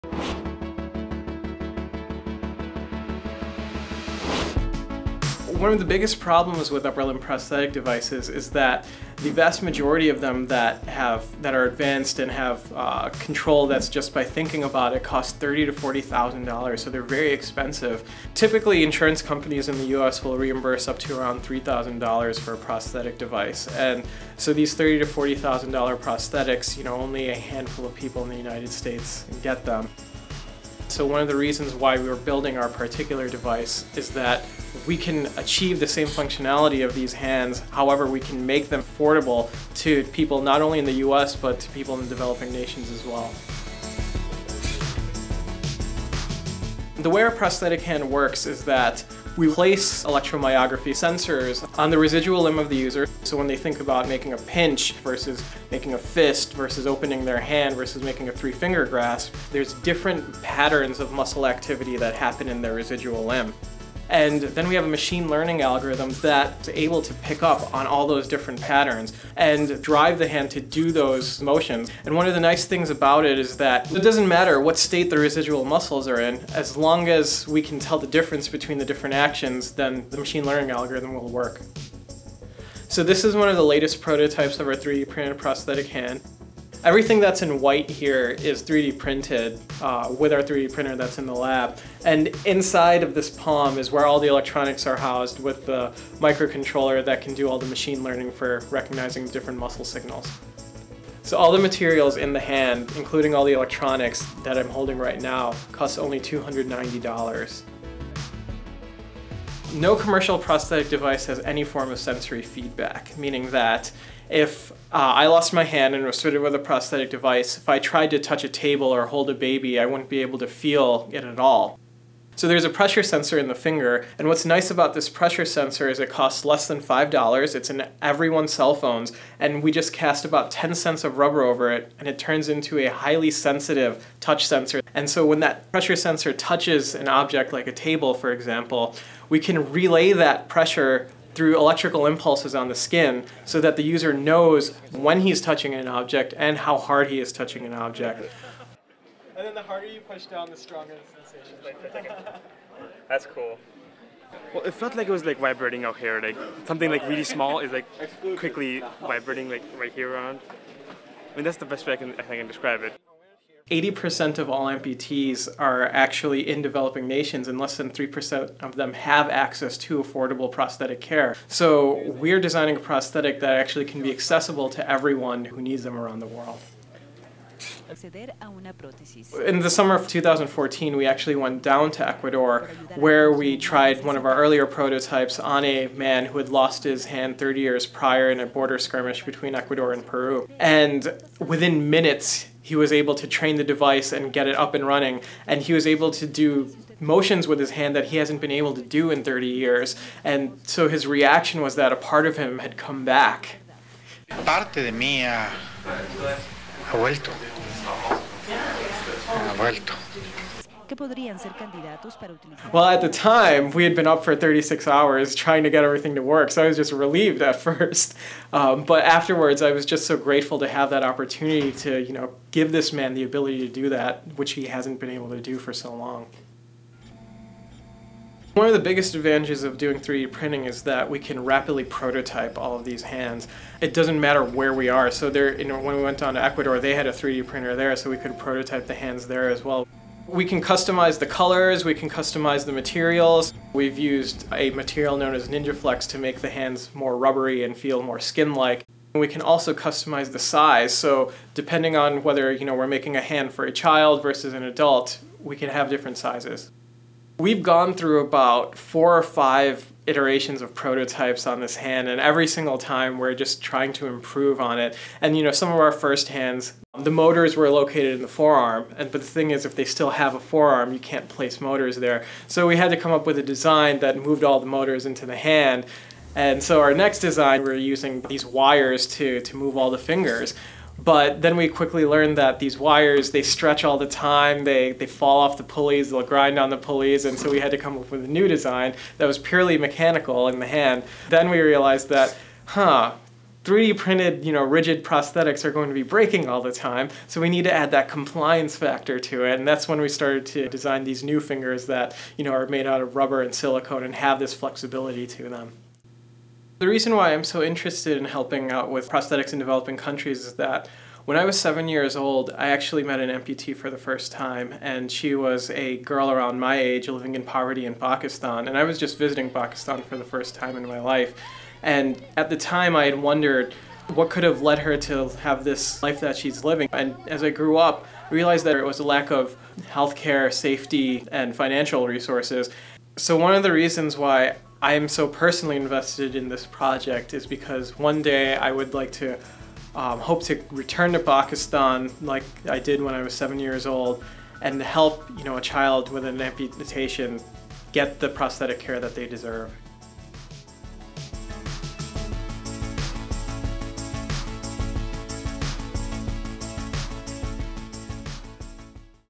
Building a Better Hand: A Low-Cost, High-Tech, 3D-Printed Prosthesis for the World
Neuroscientist Aadeel Akhtar and his colleagues have created a more functional, and far less costly, bionic hand that may revolutionize prosthetics worldwide.

Using myoelectric sensors, the fully-functional hand can be controlled by muscles in the wearer's residual limb.  And, unlike other commercial prosthetics, the hand also provides sensory feedback to the wearer.

Akhtar is a M.D./Ph.D. student in the Neuroscience program at the University of Illinois.  He is a part of Associate Professor Timothy Bretl's research group and a member of the Beckman Institute's Artificial Intelligence group.